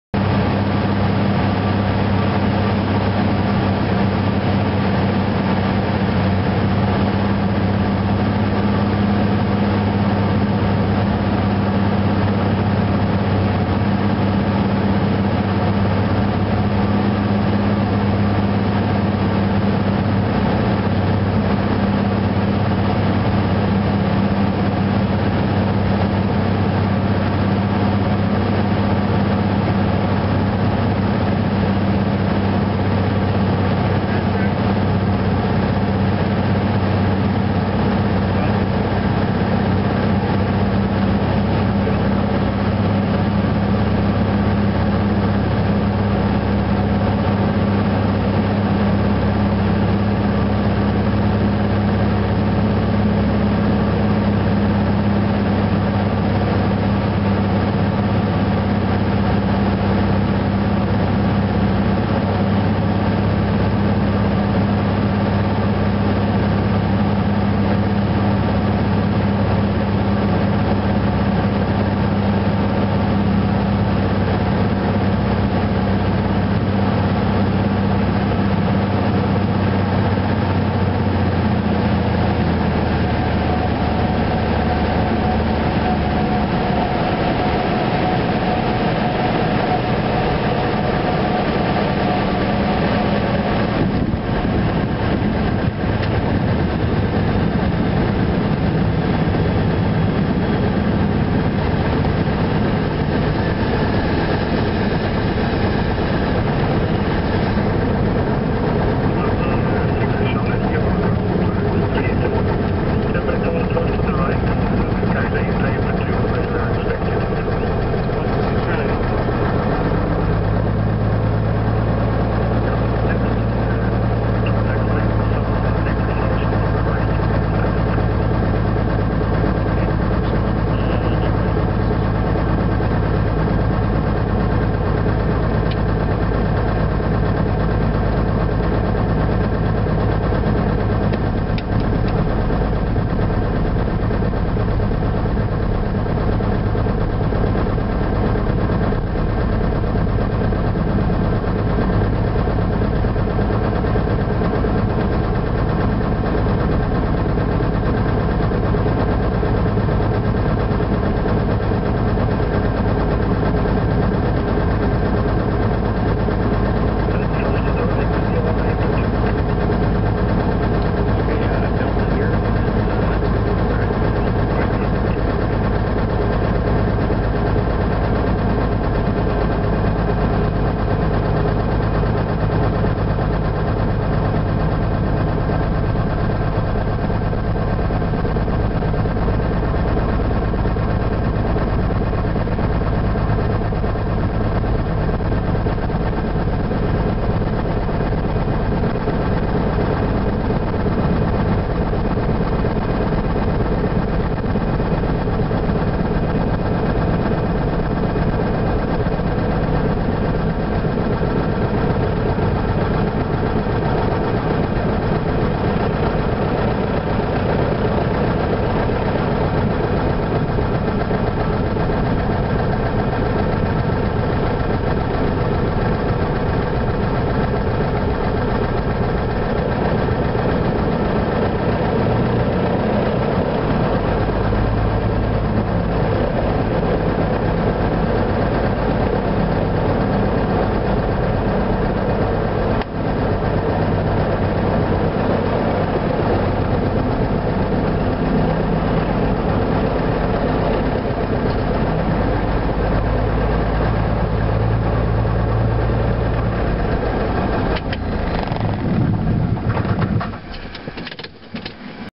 ESOW
End of flight from Minnesota to Sweden